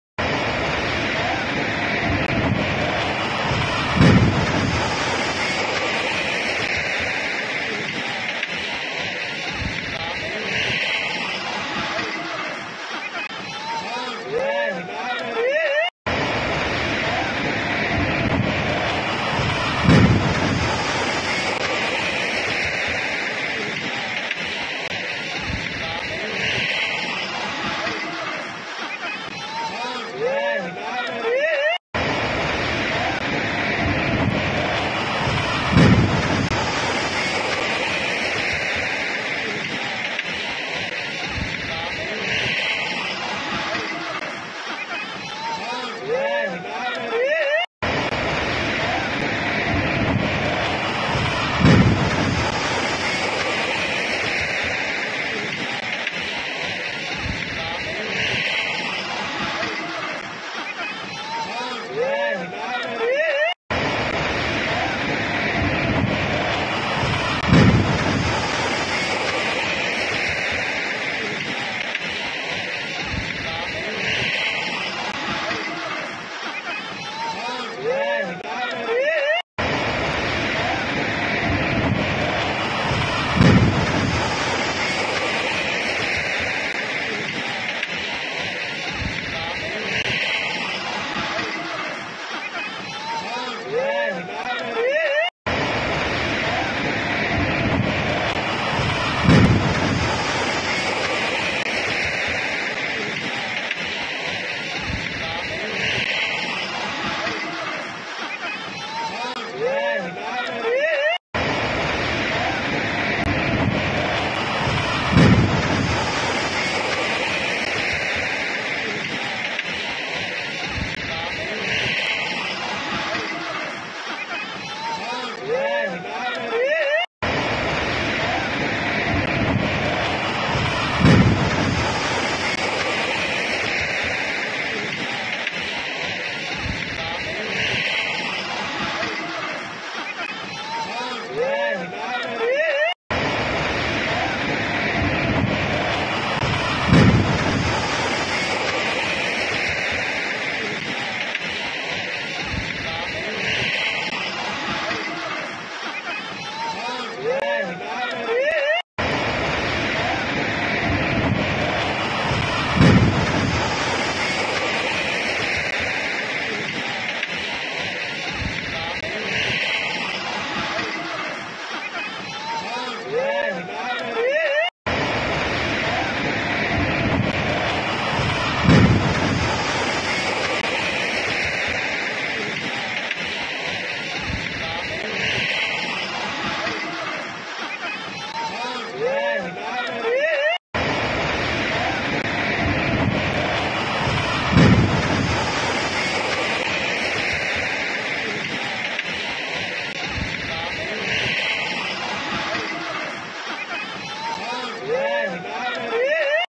Bridge Broke Bhagalpur Bihar II India
ট্ৰেইন দুৰ্ঘটনা মাৰ নাযাওঁতে এইবাৰ বিহাৰৰ ভাগলপুৰত ১৭ হাজাৰ কোটি টকা ব্যয় সাপেক্ষে নিৰ্মান কৰা এখন বৃহৎ দলং নিৰ্মাণ কাৰ্য সম্পন্ন নহওঁতেই কাৰিকৰী বিজুটি ঘটাত খহি পৰে । উক্ত ঘটনাত বৰ্তমানলৈকে কোনো লোকে হতাহঁত হোৱাৰ তথ্য প্ৰকাশ পোৱা নাই।
A huge bridge constructed at a cost of Rs 17,000 crore in Bhagalpur, Bihar, collapsed before completion due to technical failure. There were no reports of casualties in the incident.